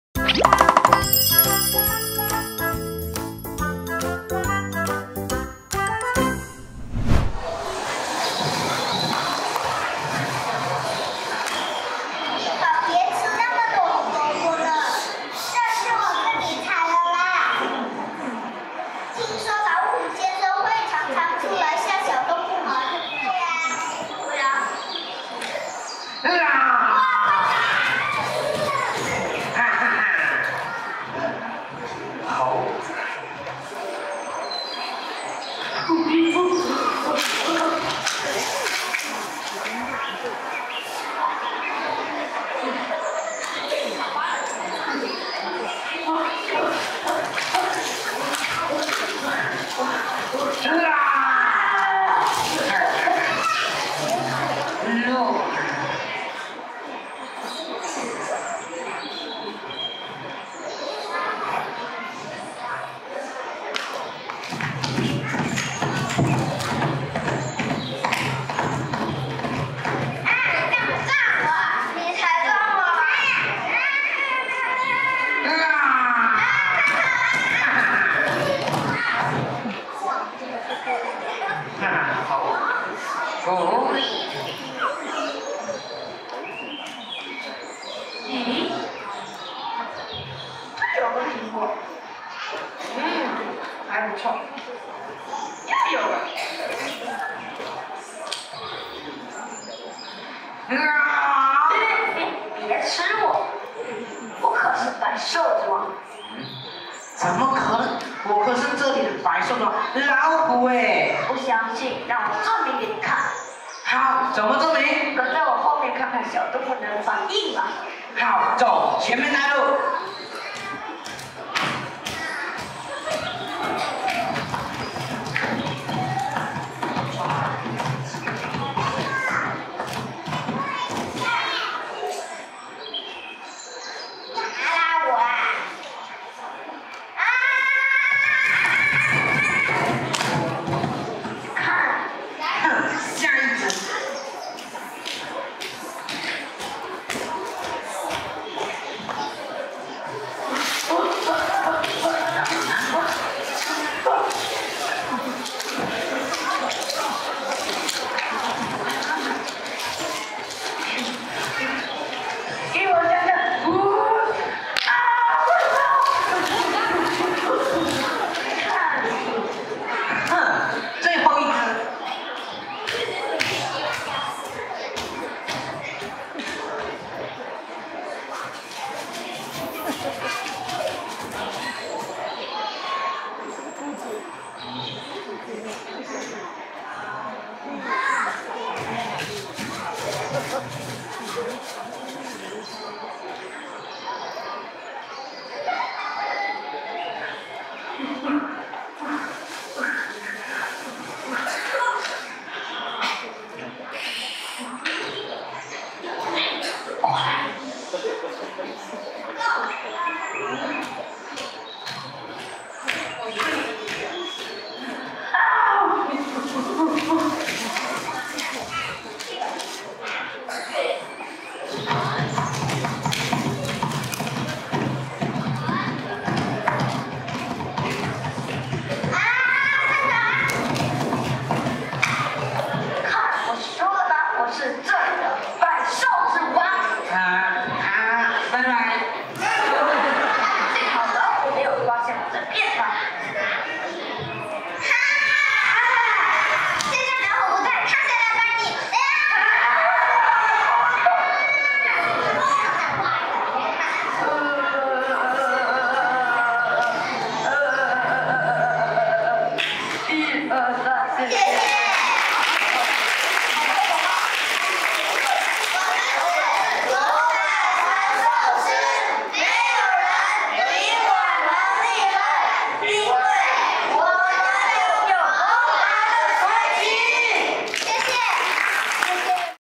《狐假虎威》- “魔法传授师” ｜2023年第19届“好玩的戏剧”儿童假日营《戏法·变变变》成果演出
2023年第19届“好玩的戏剧”儿童假日营《戏法·变变变》成果演出
剧目：《狐假虎威》
组名：“魔法传授师”
学长：陈榆桐、谢晓萱
组员：
林隽熙
萧芷芯
曾博信
黄靖依
蔡昊辰
沙晏葶
姚诚史
杨凯晴
陈卉颐
董心怡
杨凯纹
徐喡健
姚茗文